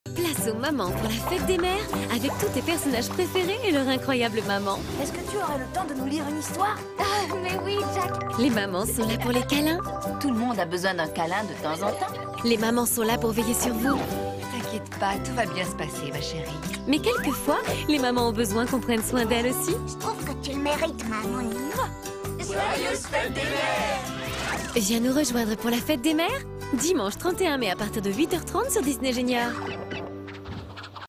Spéciale Fêtes des Mères - Dimanche 31 Mai à partir de 8h30 sur Disney Junior !
Retrouvez la journée spéciale Fêtes des Mères le Dimanche 31 Mai à partir de 8h30 sur Disney Junior.